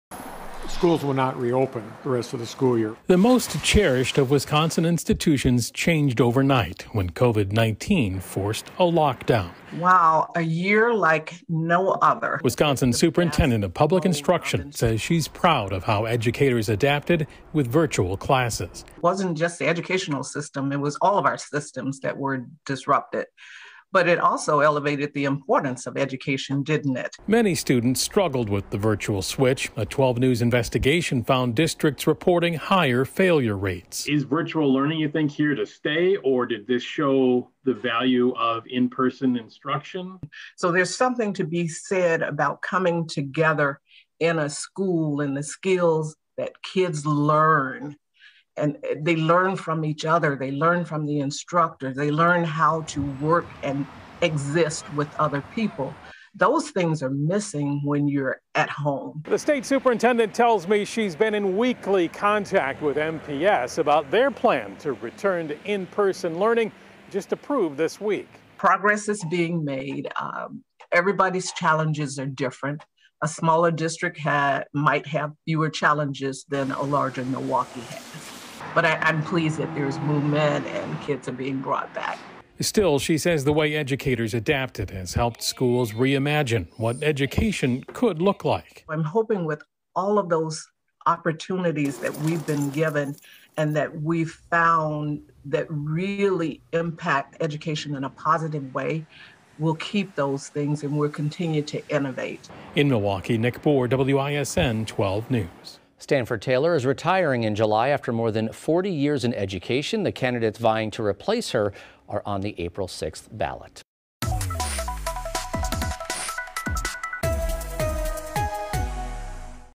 State superintendent says in-person school important
Carolyn Stanford Taylor says working with others is missing from virtual learning.